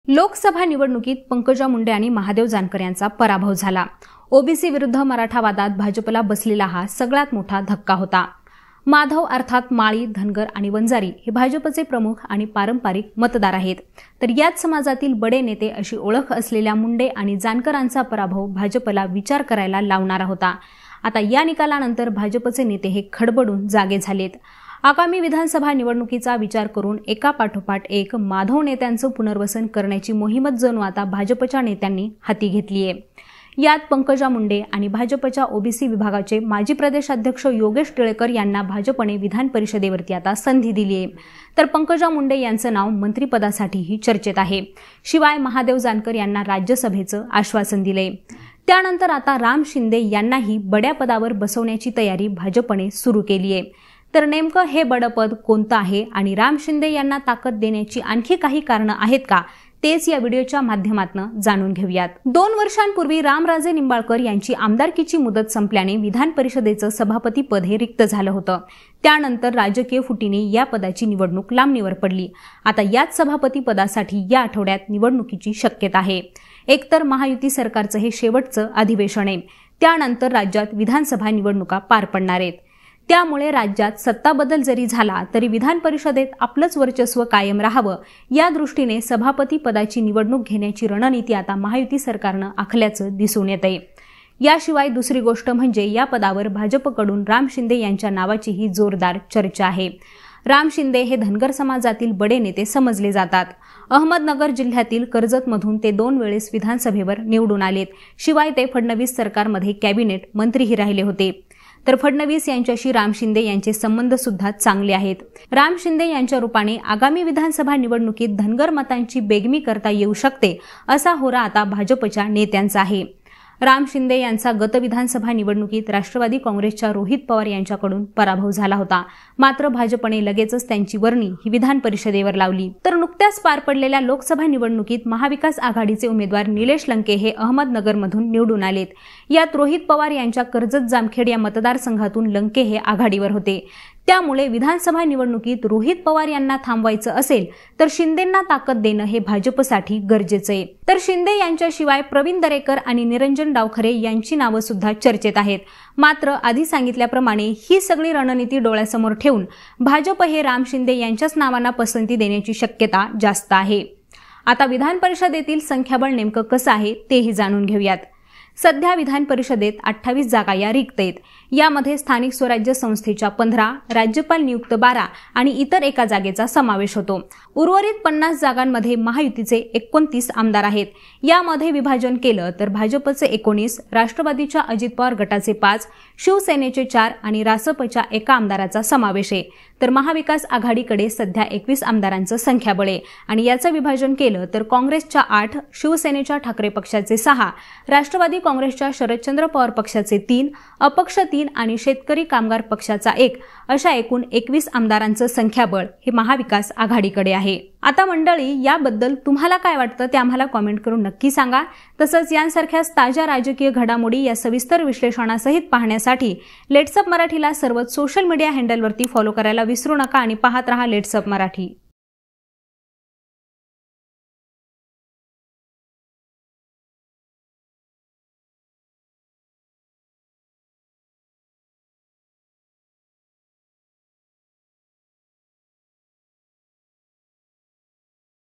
Ram Shinde : राम शिंदेंच पुनर्वसन; सभापतीपदी वर्णी लागणार? | LetsUpp Marathi
दोन वर्षांपूर्वी रामराजे निंबाळकर यांची आमदारकीची मुदत संपल्याने विधान परिषदेच सभापतीपद रिक्त आहे. या पदासाठी भाजकडून राम शिंदे यांच्या नावाची चर्चा सुरू आहे. विधानसभा निवडणुकीनंतर राज्यात सत्ताबदल झाला तरी विधान परिषदेत आपले वर्चस्व कायम राहावे या दृष्टीने सभापती पदाची निवडणूक घेण्याची रणनीती महायुती सरकारने आखली आहे. यासाठी राम शिंदे यांची वर्णी लागू शकते. याद्दलच सविस्तर जाणून घ्या...